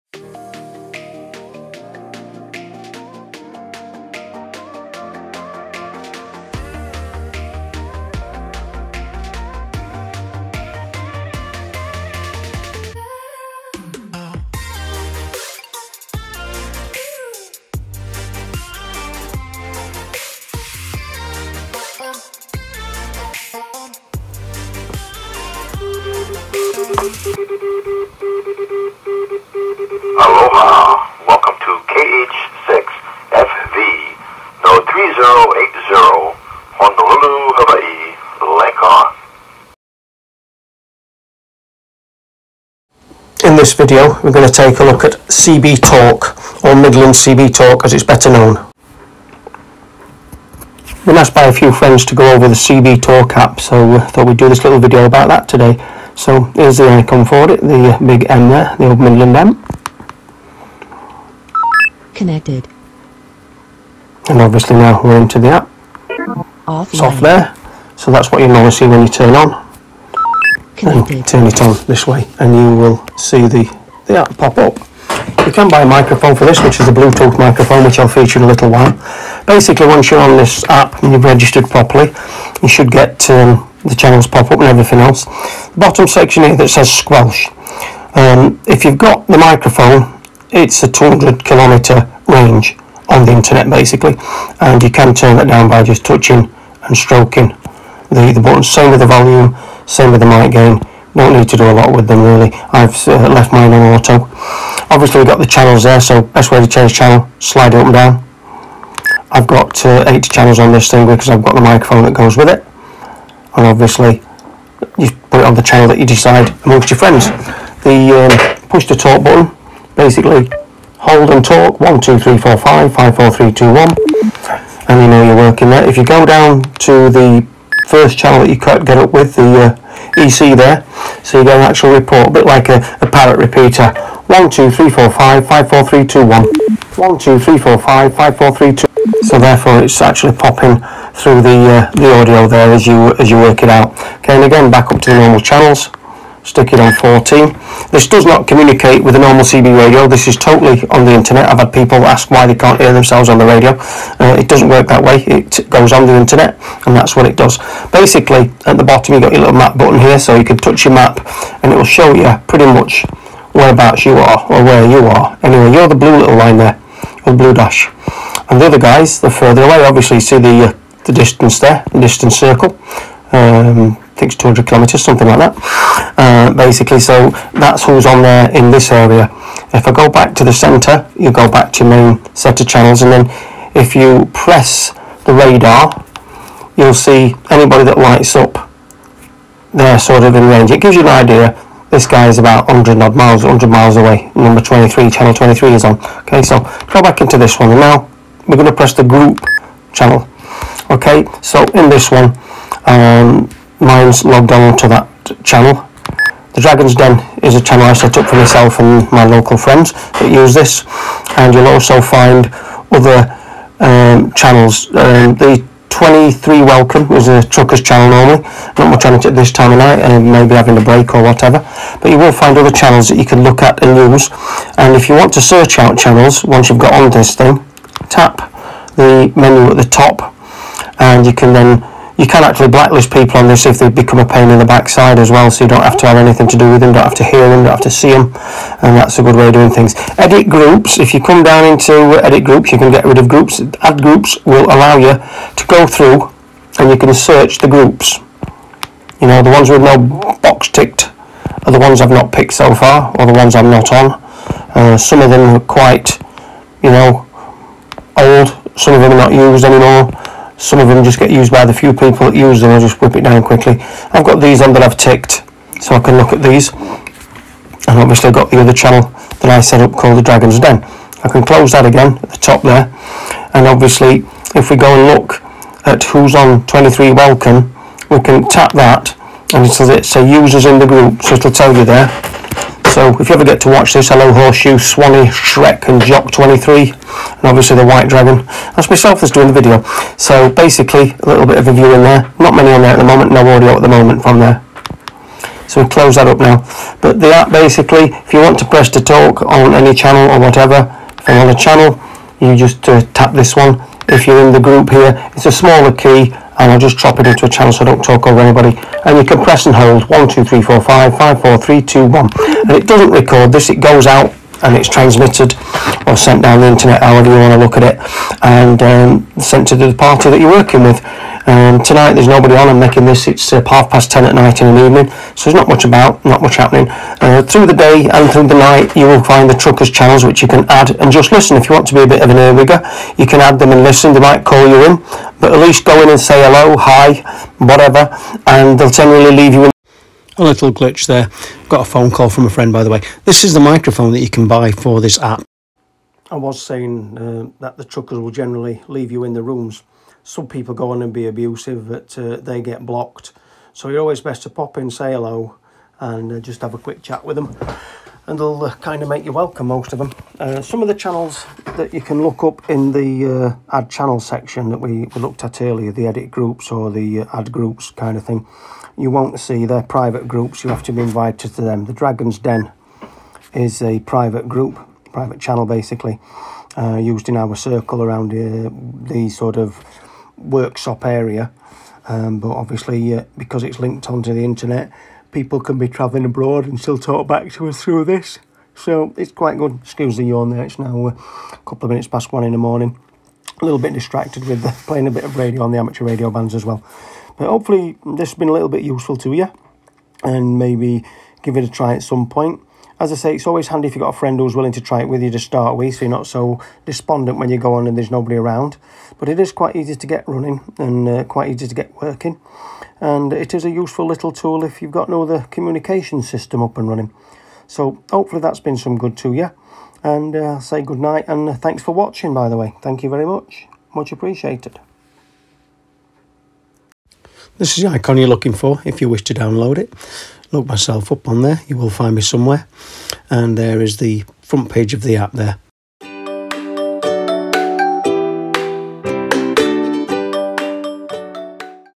Midland CB Talk app
A quick run through of the basics of the Midland CB Talk app for android and IPhone. A network radio app, that allows you to use your phone like a CB radio. A handy free app that could help you keep in touch with family and friends, a handy communication app

Audio may be a little bit rough. Filming from the screen of my phone I was unable to control sound volume as easily as in the app I use to make the videos.